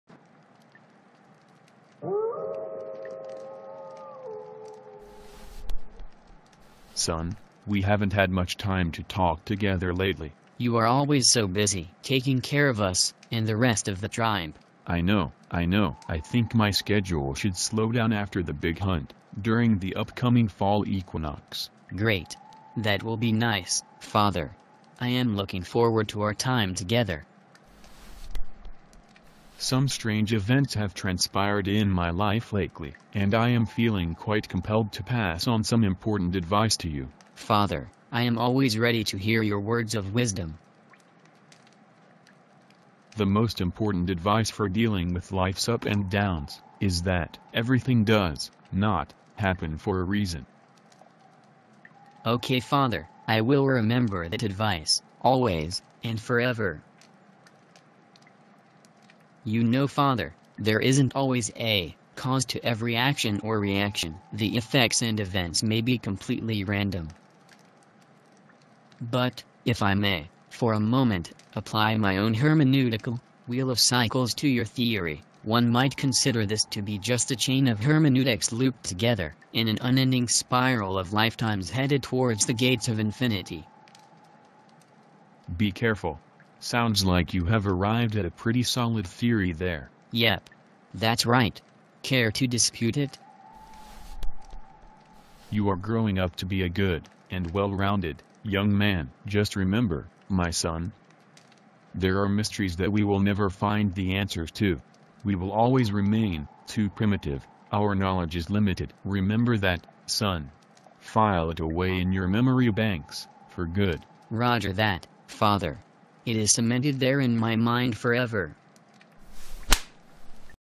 PRIMITIVE (Scenes From The Neanderthal World)
PRIMITIVE
(Scenes From The Neanderthal World)

This is a Father and Son Talk from the ancient world.